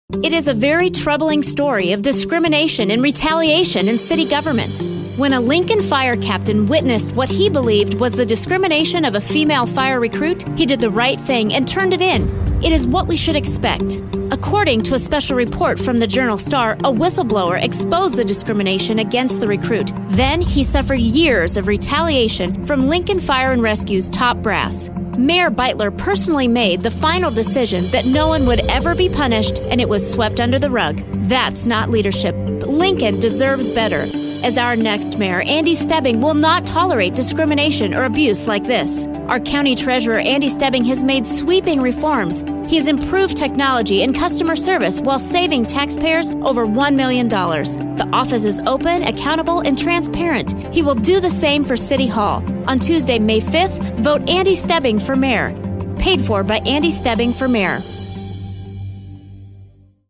Transparency | Stebbing for Mayor Radio
It is a very troubling story of discrimination and retaliation in city government.

When a Lincoln fire captain witnessed what he believed was discrimination of a female fire recruit, he did the right thing and turned it in. It is what we should expect.

According to a special report from the Journal Star, a whistle-blower exposed the discrimination against the recruit - then he suffered years of retaliation from Lincoln Fire and Rescue’s top brass.

Mayor Beutler personally made the final decision that no one would ever be punished and it was swept under the rug.

That’s not leadership. Lincoln deserves better.

As our next mayor, Andy Stebbing will not tolerate discrimination or abuse like this. Our County Treasurer Andy Stebbing has made sweeping reforms. He has improved technology and customer service while saving taxpayers over 1 million dollars. The office is open, accountable and transparent. He will do the same for city hall.

On Tuesday May 5, vote Andy Stebbing for Mayor. Paid for by Andy Stebbing for Mayor